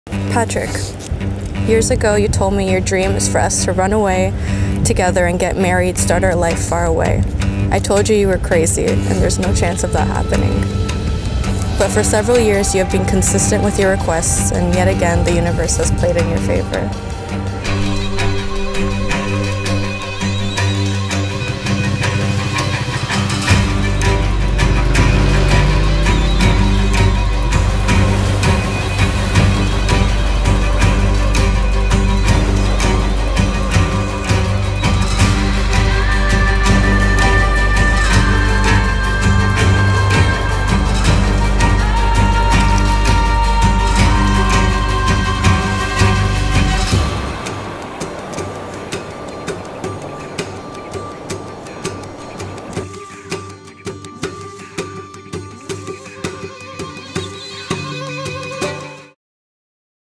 Tofino Elopement Wedding | Vancouver Island BC
Beautiful teaser of an elopement in Tofino British Columbia!